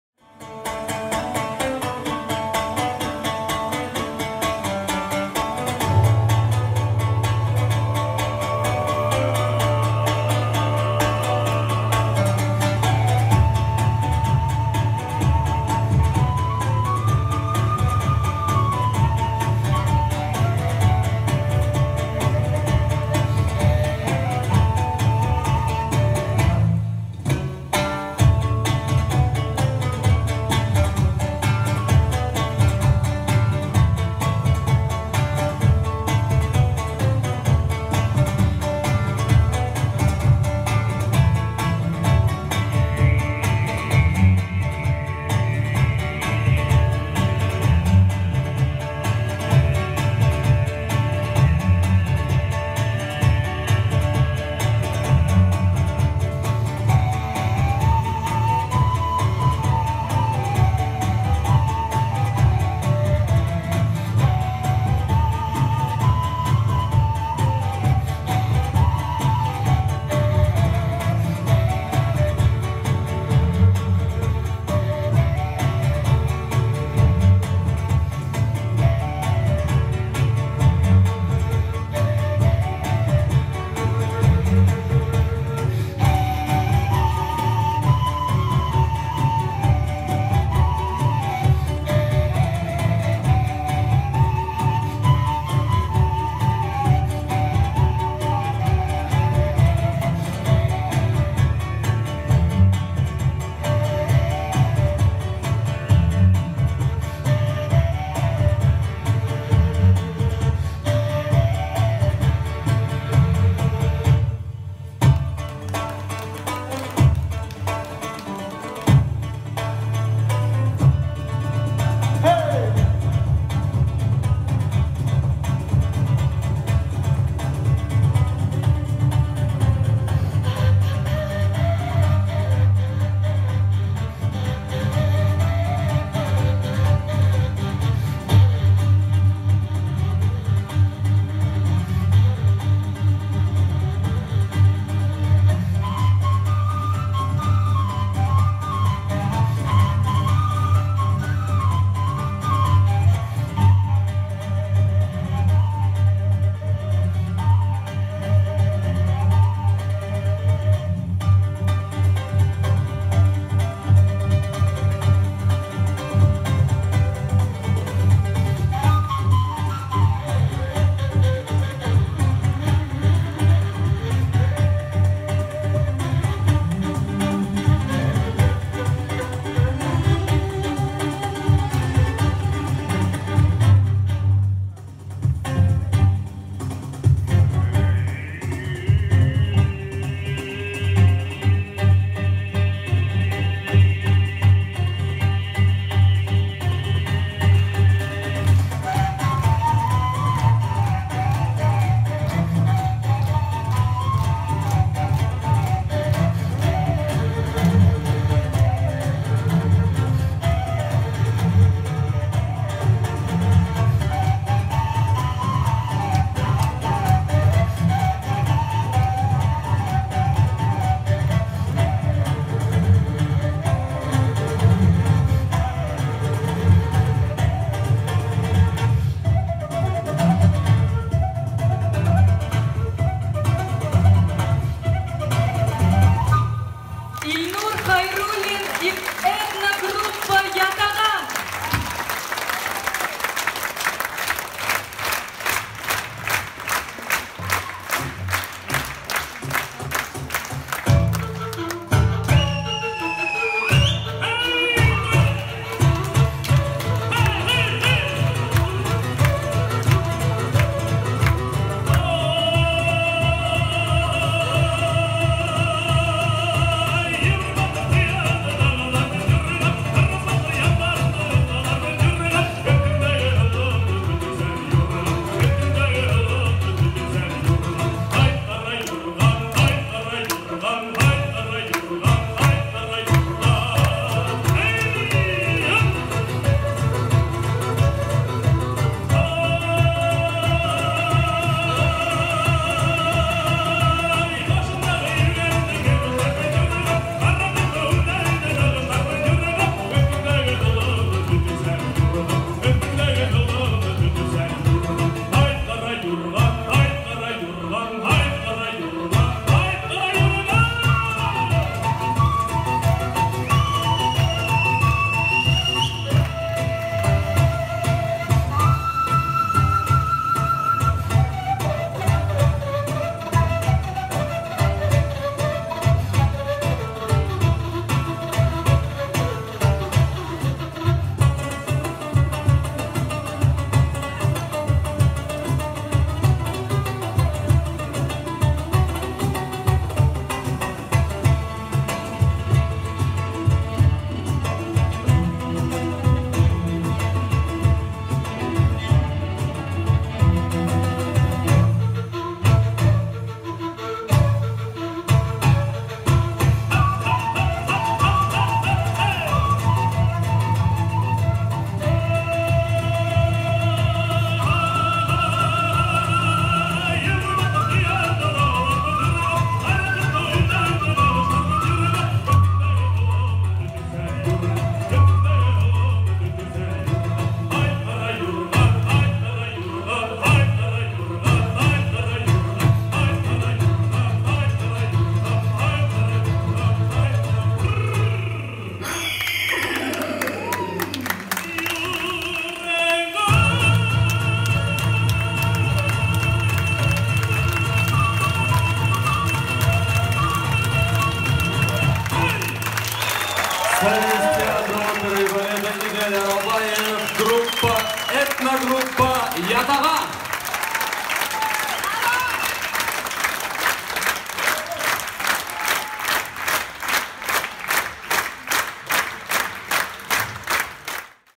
Этно-концерт в театре башдрамы | Этно-группа Йатаган
В рамках театрального фестиваля "Туганлык" состоялся этно-концерт, где была показана культура башкирского, тувинского, калмыцкого, кыргызского и др.народов.
Уфа, 10 сентября, 2017 года
Этно-группа йатаган (Республика Башкортостан)